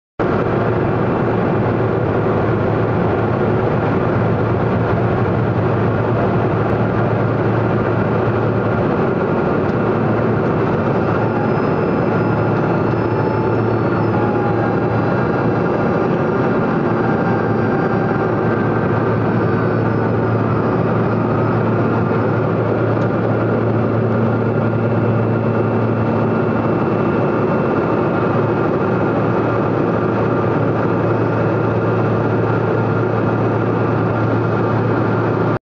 Pilatus PC-12 Night Takeoff
Night Takeoff in the Pilatus Runway 31 KBTR.